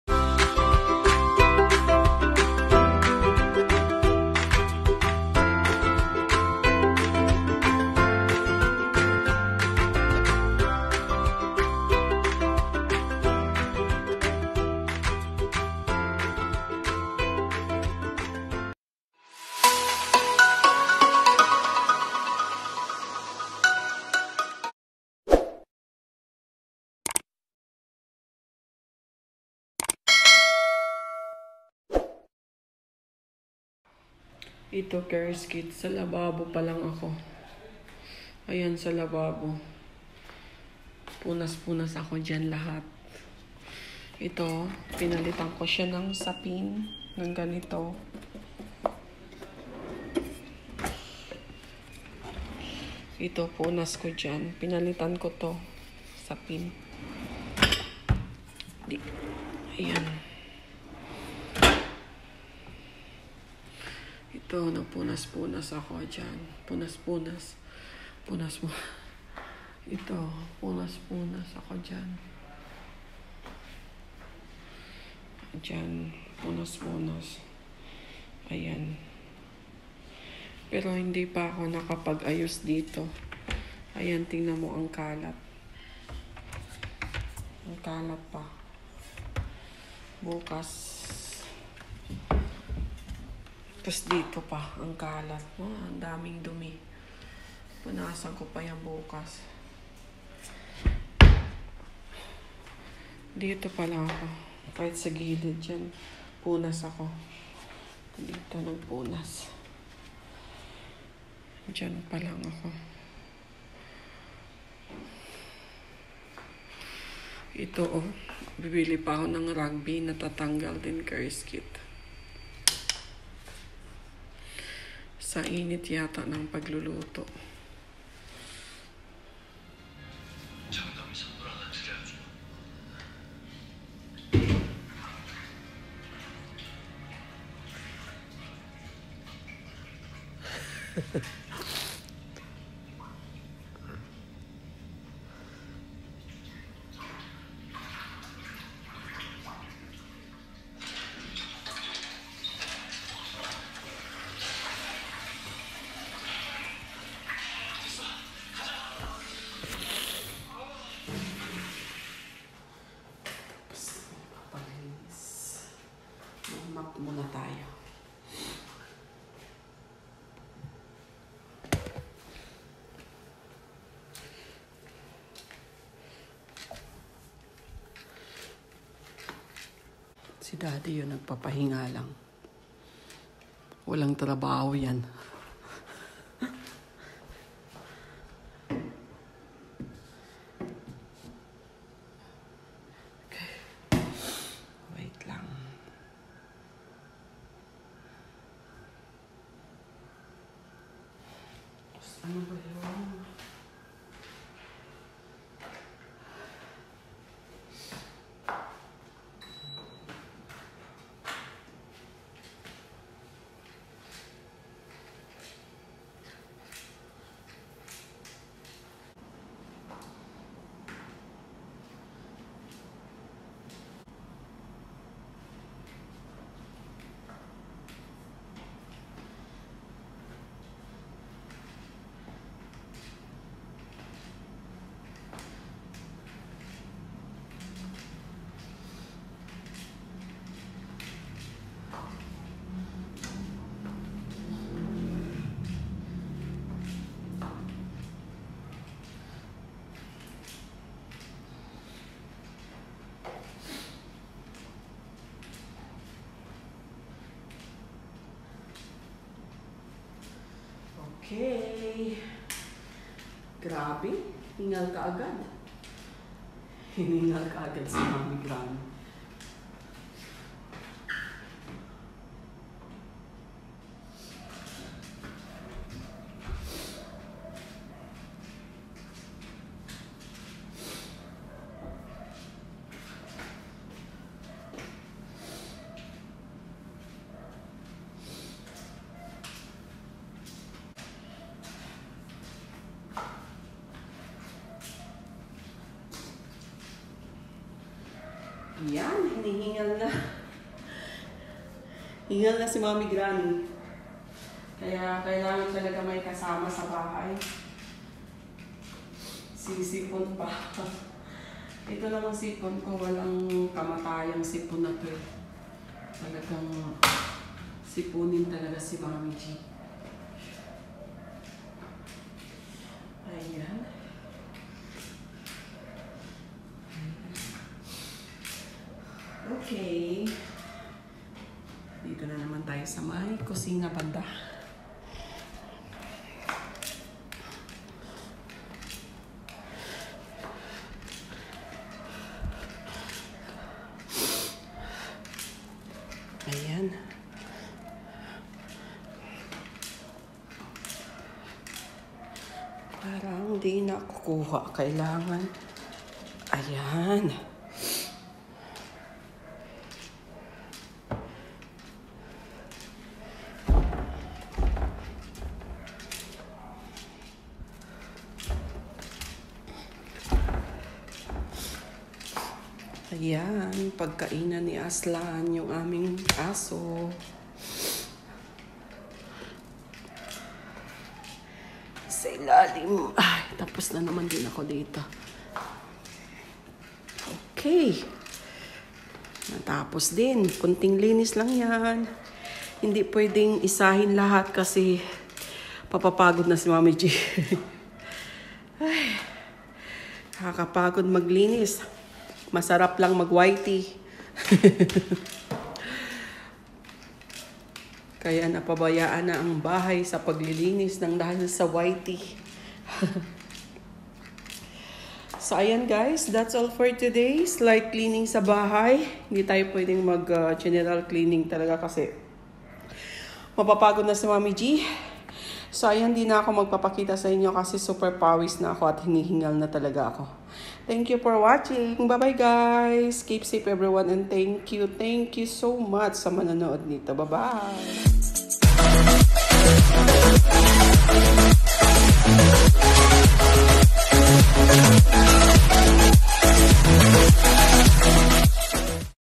Konting linis lang at nakkapagod na talaga, ganito nb pag nag kkaedad na
Konting linis lang at nakkapagod na talaga, ganito nb pag nag kakaedad na @mommygrannysvlog3050